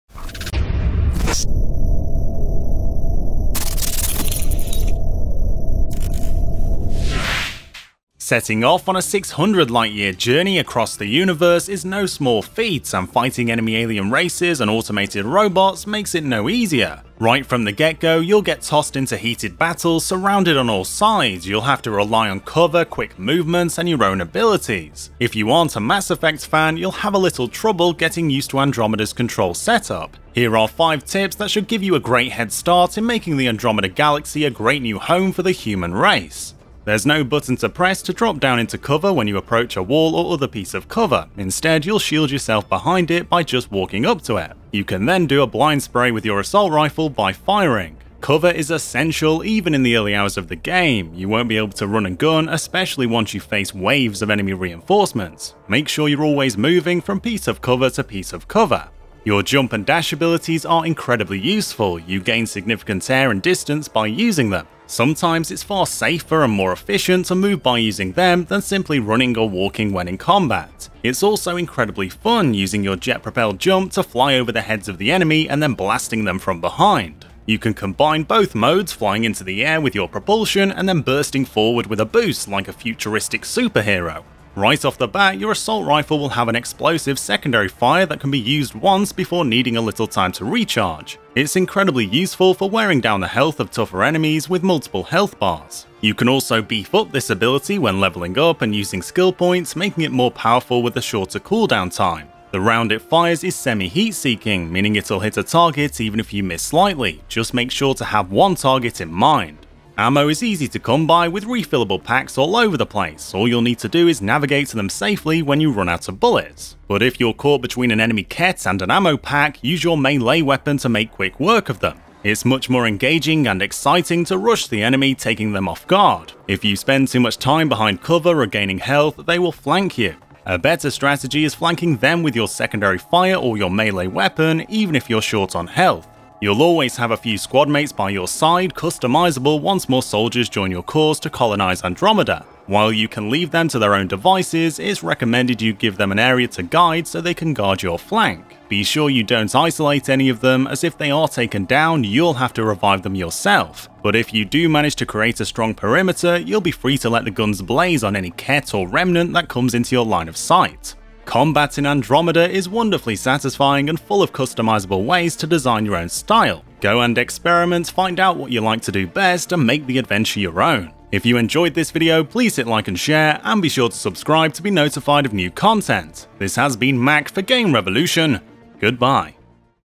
5 Tips I Wish I Knew Before Starting Mass Effect: Andromeda
Mass Effect: Andromeda isn't an easy game. These 5 quick combat tips will go a long way toward keeping you alive.